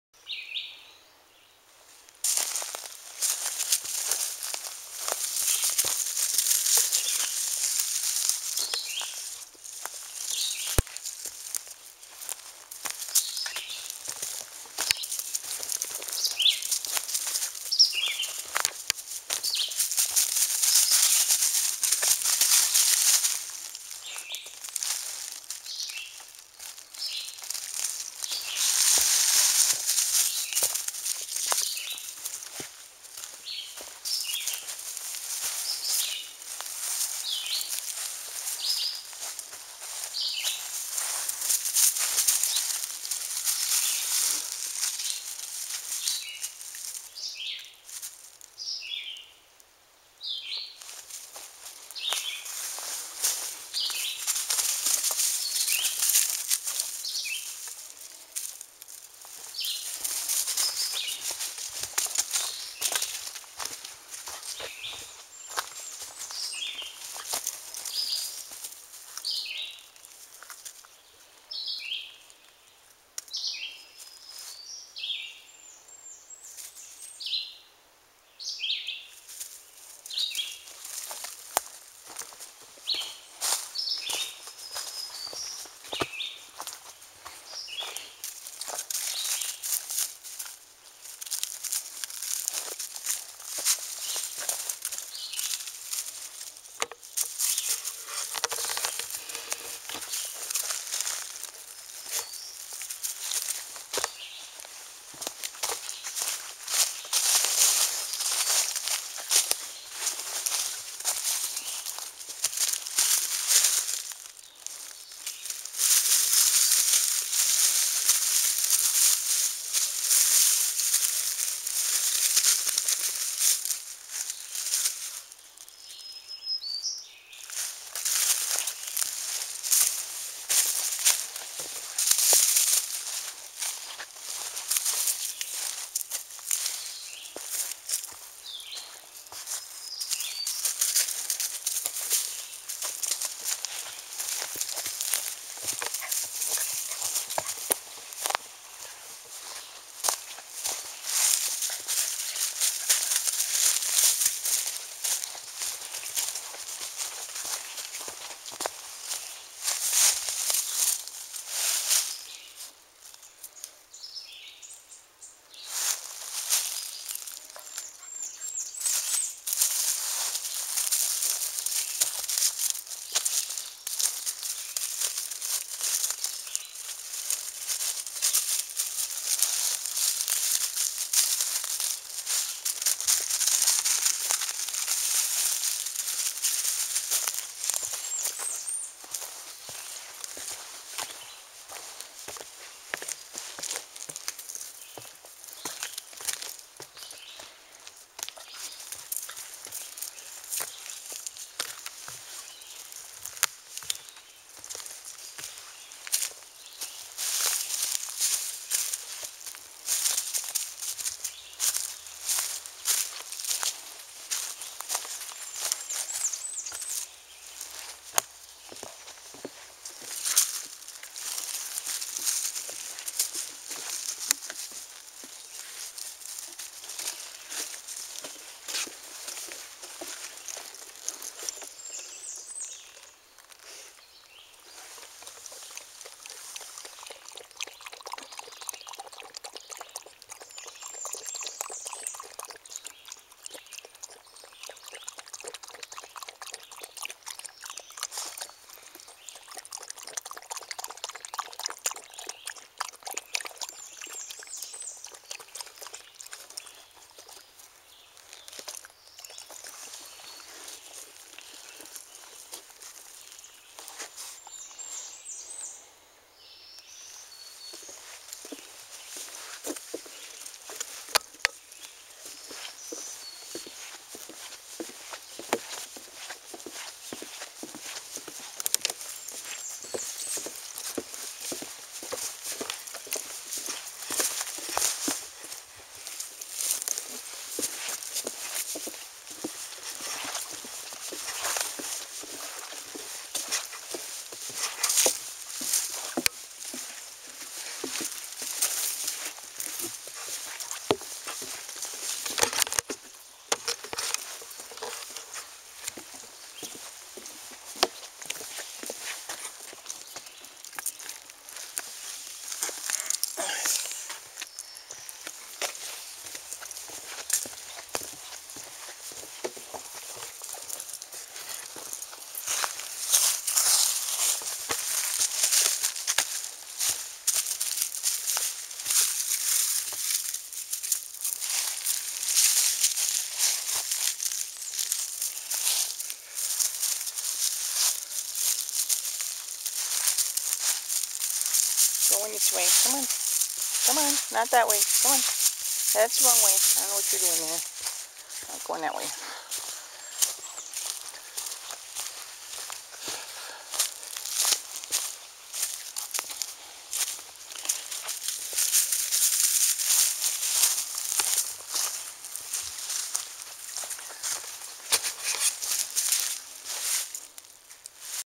Penny's First Hike at Dyken Pond EEC
The first of many more hikes with our new beagle puppy, Penny!